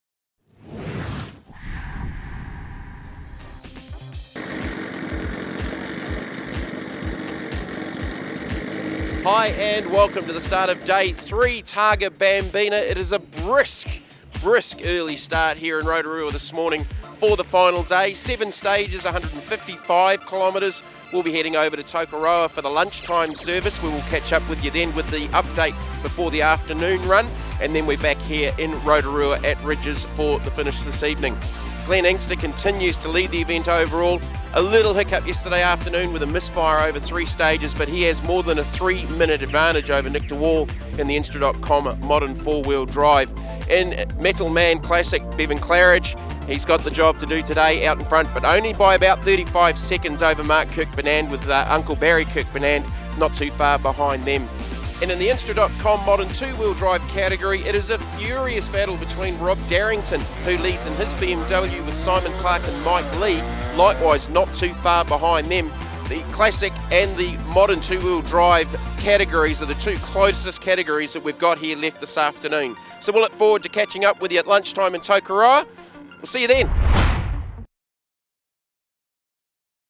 Day 3 Starts- Targa Bambina 2015
Jamie McCarthy brings you all the news ahead of the third and final day from Targa Bambina 2015